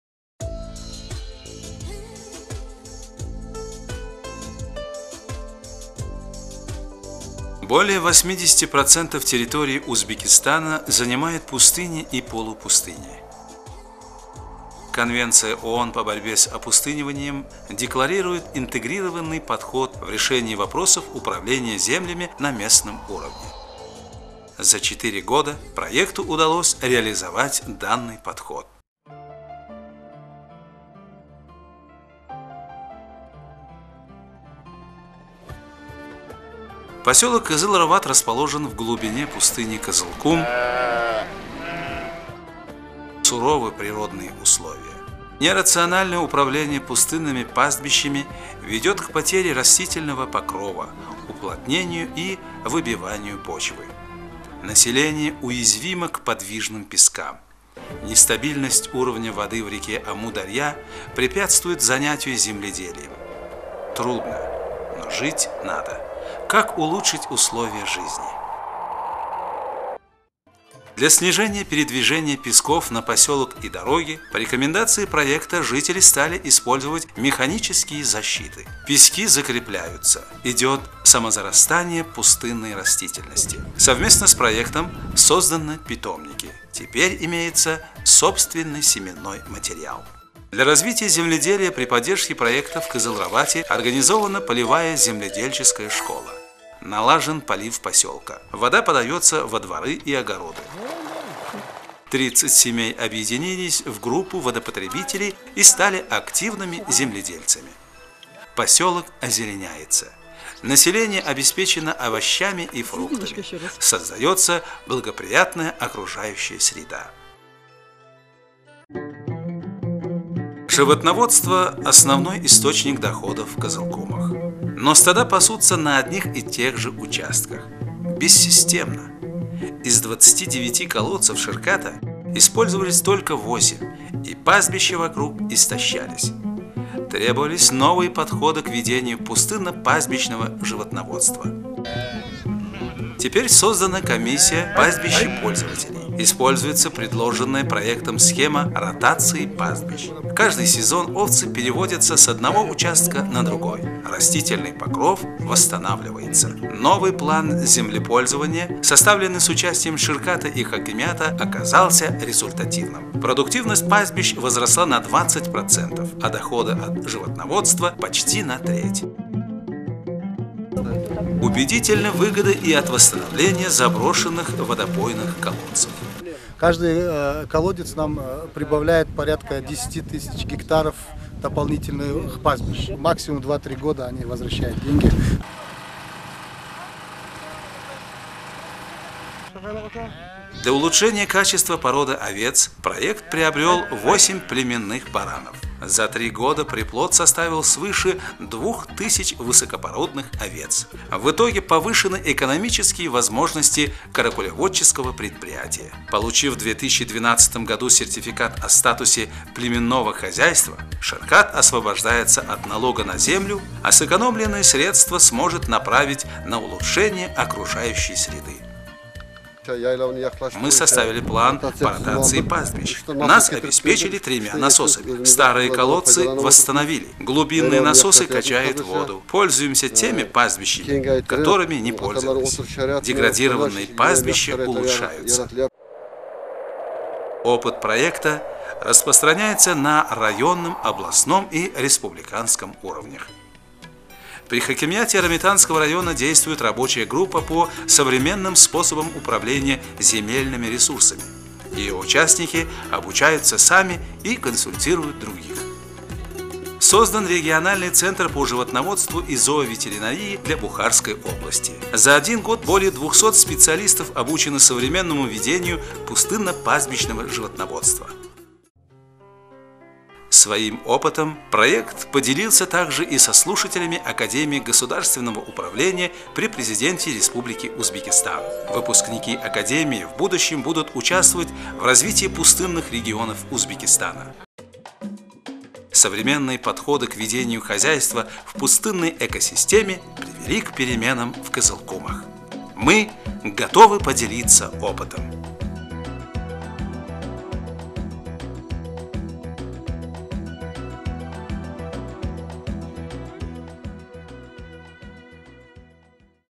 CHANGES IN THE KYZYLKUM ( UNDP/GEF SLM project UZB)
UNDP/GEF "Achieving ecosystem stability on degraded land in Karakalpakstan and Kyzylkum desert" project (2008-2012)

Project Manager: Ms. Irina Bekmirzaeva